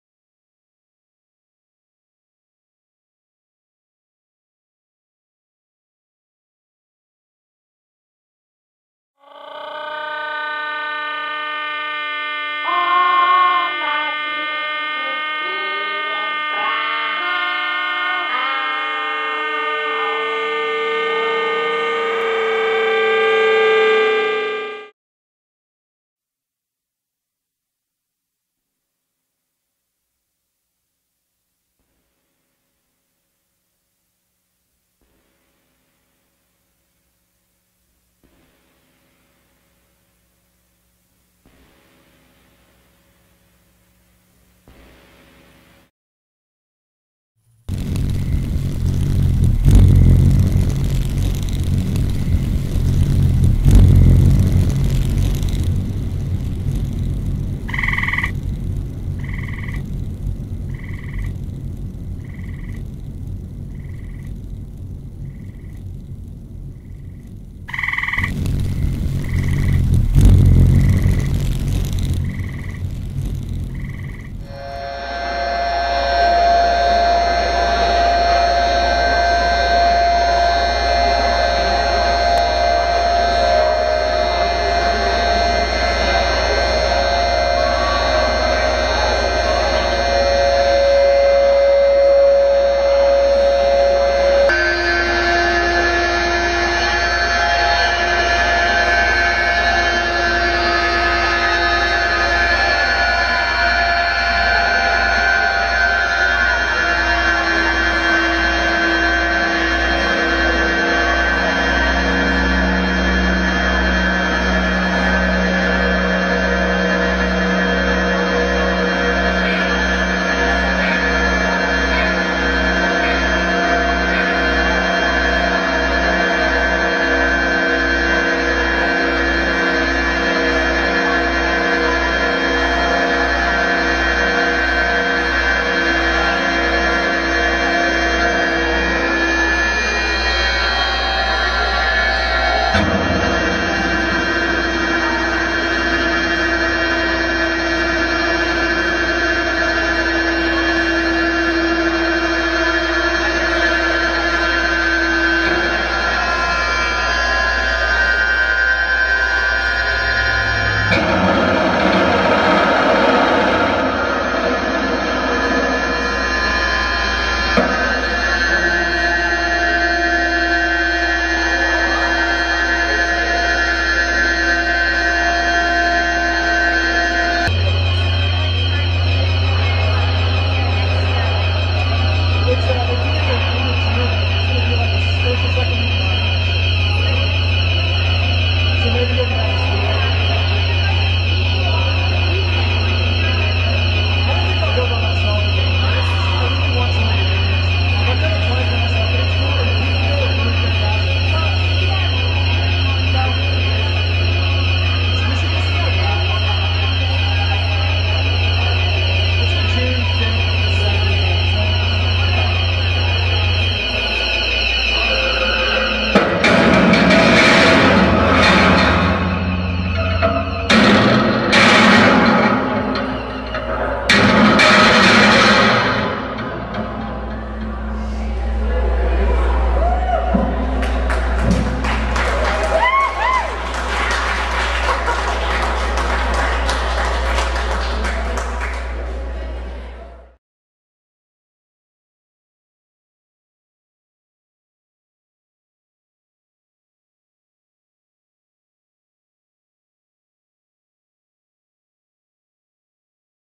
Justin Boyd Preview.m4v
Preview of Cell Series featured artist, Justin Boyd.  Check out the OJAC website for plenty of information on Boyd and the upcoming exhibition.  This video is merely examples of previous installations with selections of audio clips included in those installations.